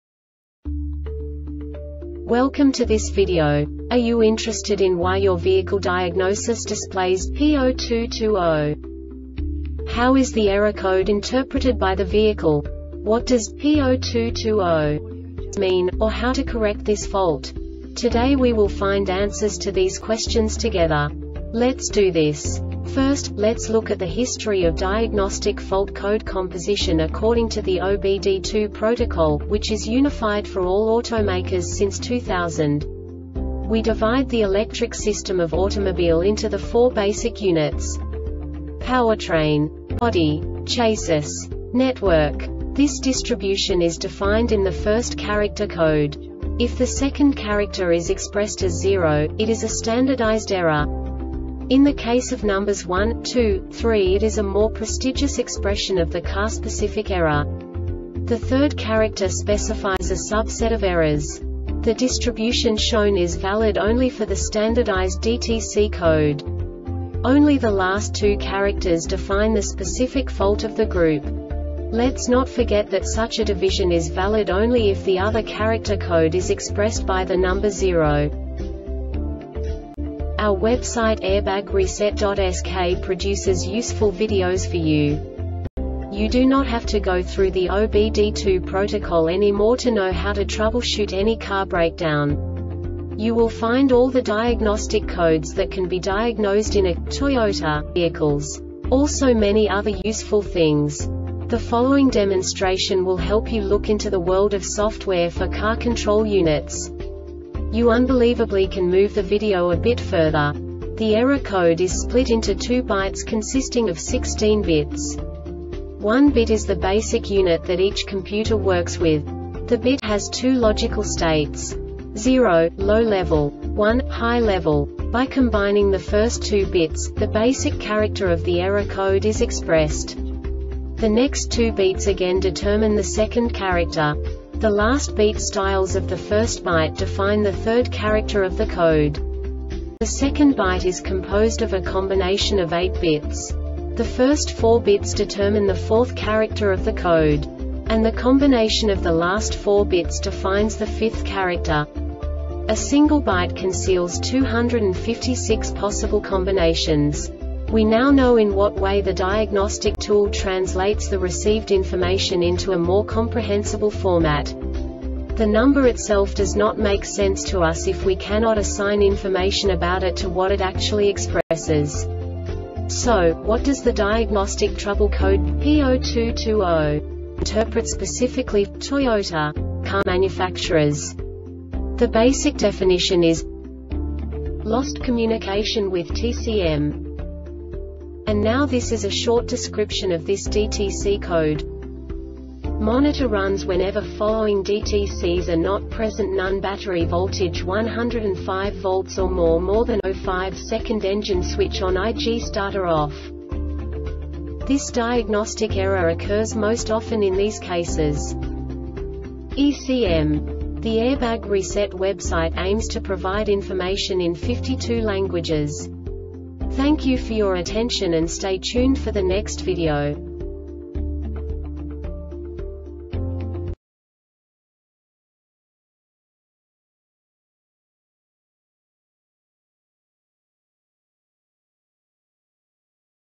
DTC Toyota P0220 Short Explanation
Contents:
0:21 Basic DTC analysis according to OBD2 protocol standard.
1:48 Insight into programming
2:58 A diagnostically understandable description of the Diagnostic Trouble Code
3:05 Basic error definition

Make:
Toyota
Code:
P0220
Definition:
Lost Communication with TCM
Description:
Monitor runs whenever following DTCs are not present: None Battery Voltage: 10.5 V or more (More than 0.5 second) Engine switch: On (IG) Starter: OFF
Cause:
ECM